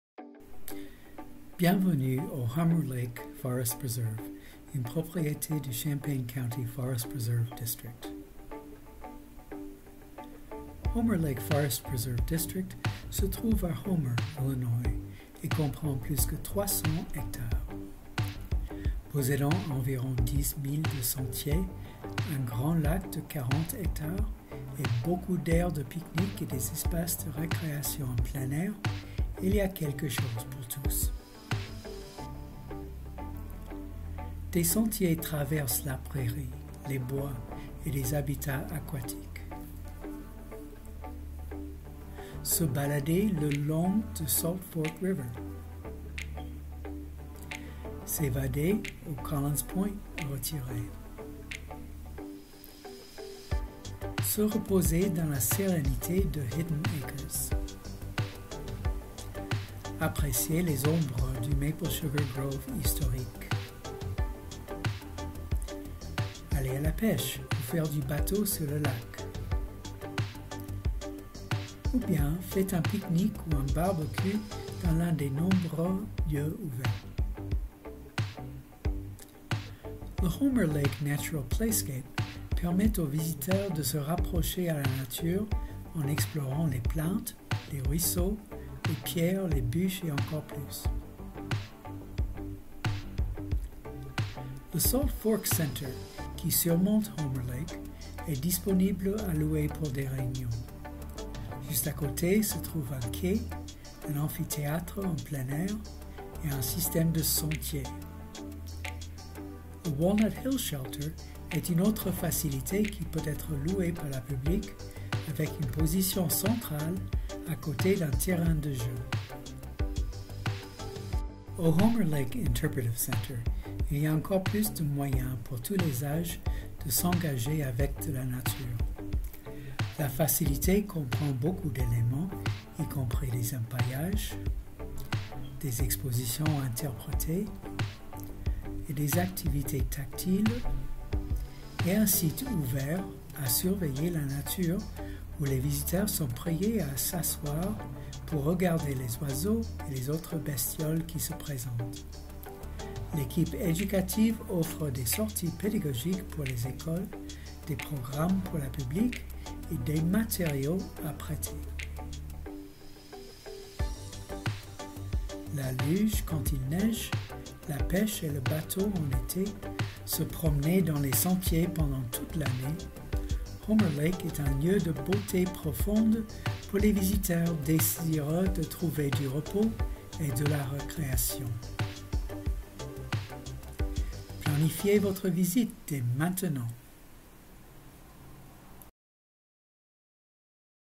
Bienvenue à Homer Lake Forest Preserve
Homer Lake Forest Preserve est l'une des six réserves du district de la réserve forestière du comté de Champaign.  Dans cette courte vidéo, nous vous présentons certaines des attractions les plus populaires de Homer Lake.  Préparez votre visite dès maintenant!

This virtual event is part of the Welcoming Champaign County Collaborative in cooperation with the New American Welcome Center at the University of Illinois YMCA. For more information about Welcoming Week, visit WelcomingAmerica.org.